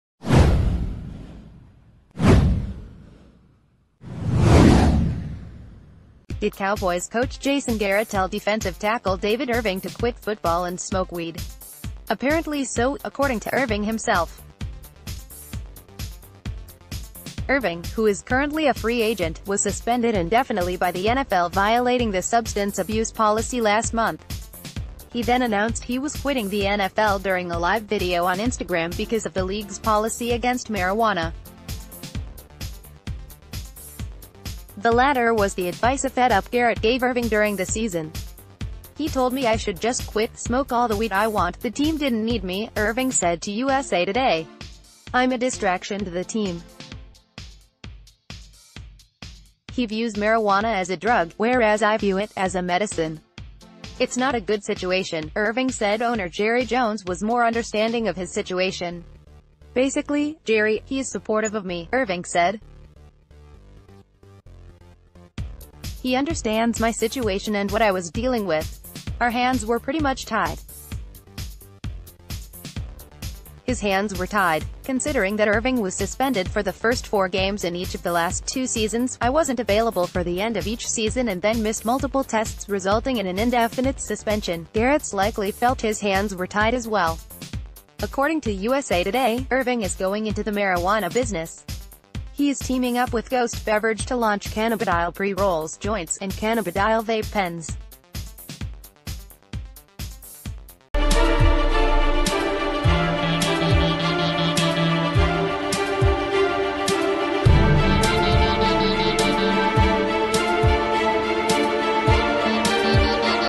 Did coach Jason Garrett tell David Irving Cowboys didn’t need him; retire, smoke weed
Did Cowboys coach Jason Garrett tell defensive tackle David Irving to quit football and smoke weed? Apparently so, according to Irving himself. Irving, who is currently a free agent, was suspended indefinitely by the NFL violating the substance abuse policy last month. He then announced he was quitting the NFL during a live video on Instagram because of the league’s policy against marijuana. The latter was the advice a fed-up Garrett gave Irving during the season. “He told me I should just quit,...